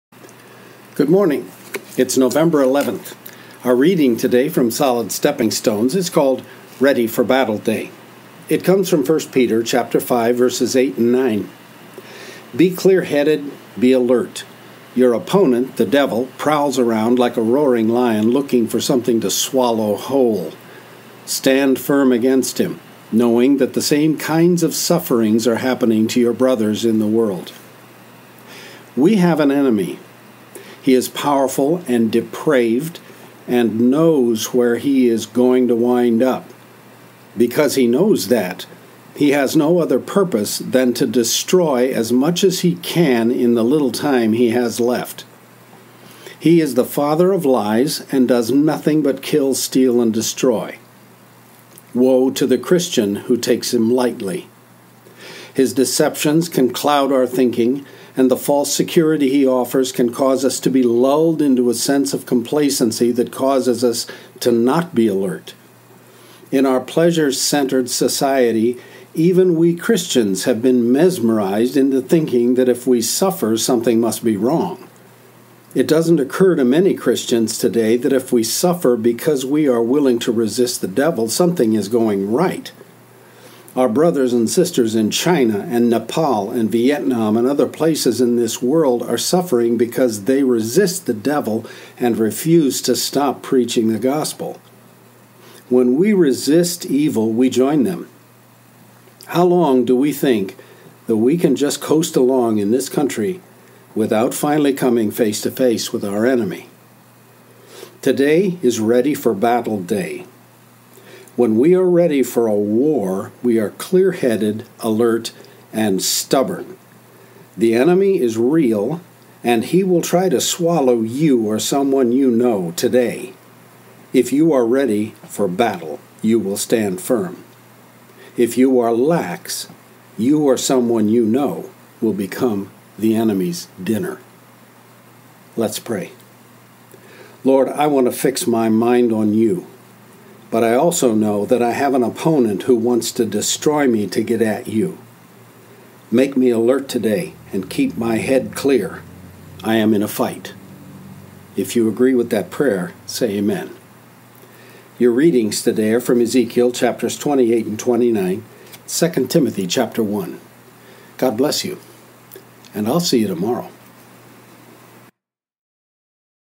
Solid Stepping Stones November 11 - Ready For Battle Day
Meditation on 1 Peter 5:8-9 from "Solid Stepping Stones" a Daily Devotional.
Photos of interesting surface textures on the Mendenhall Glacier in Juneau, Alaska.